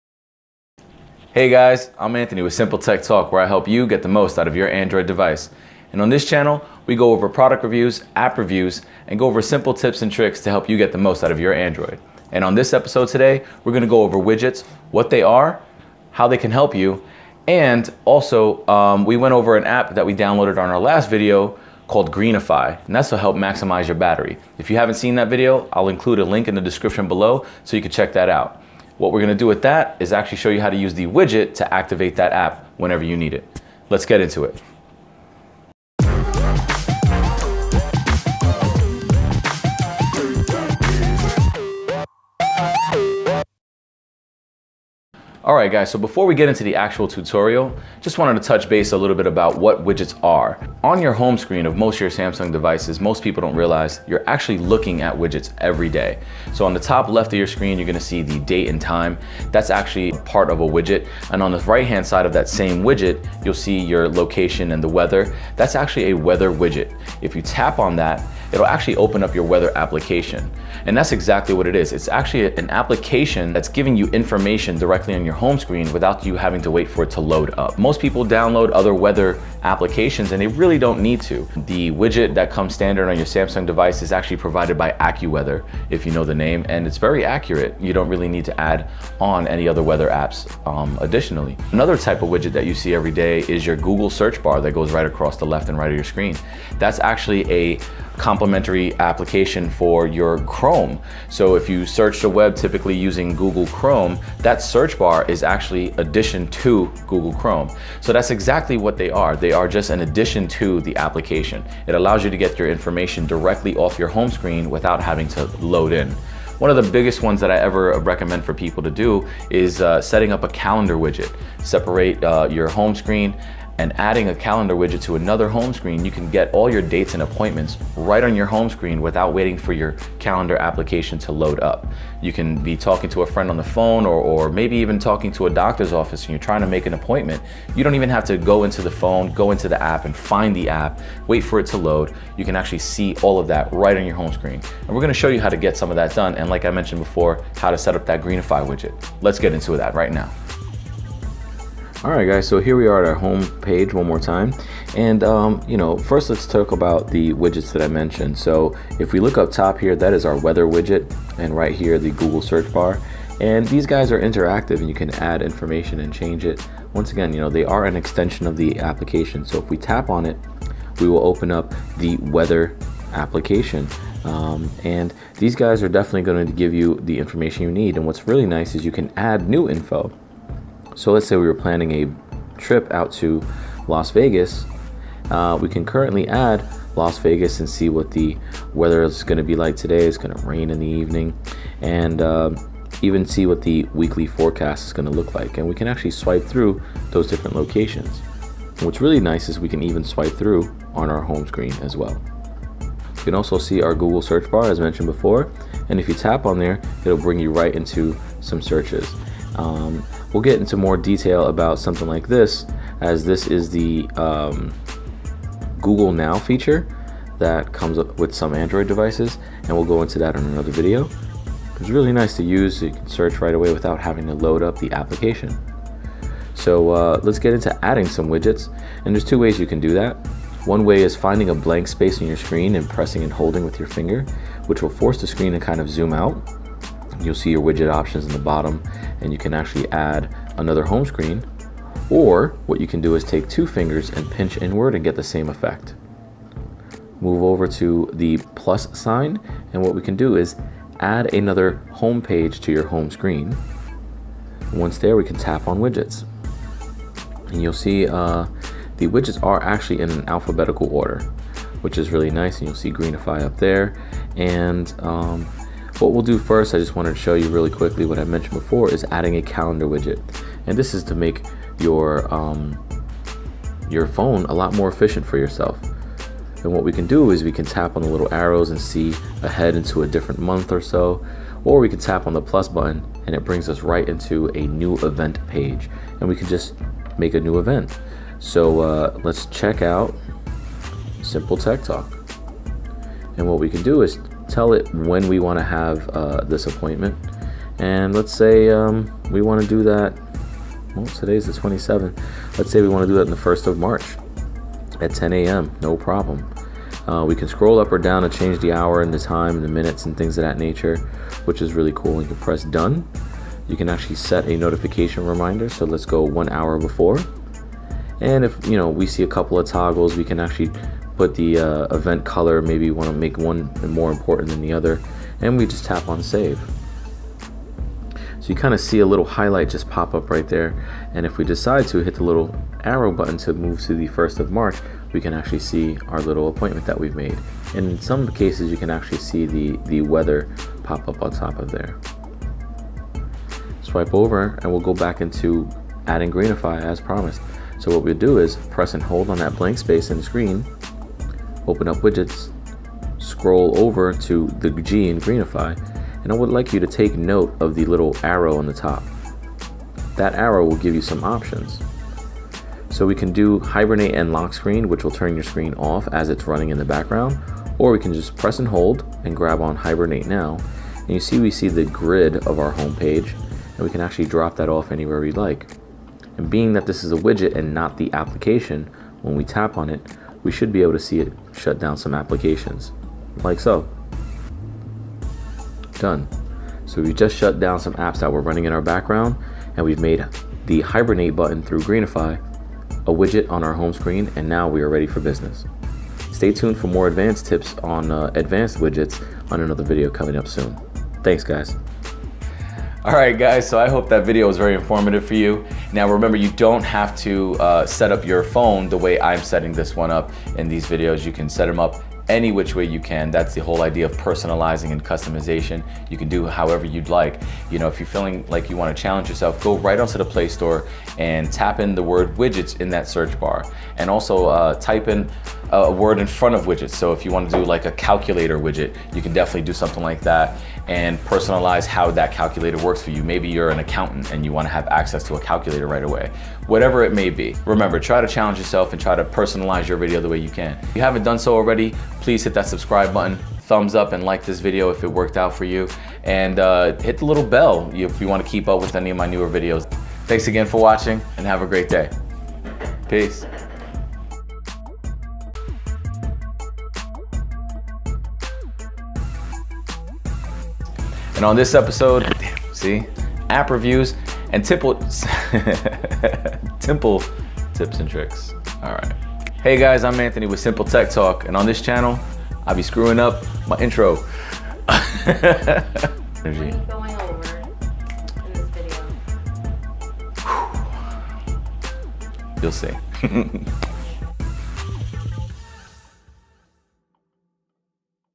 How to set up widgets - what are widgets?
Here we'll go over setting up widgets and setting up our Greenify widget from our last video.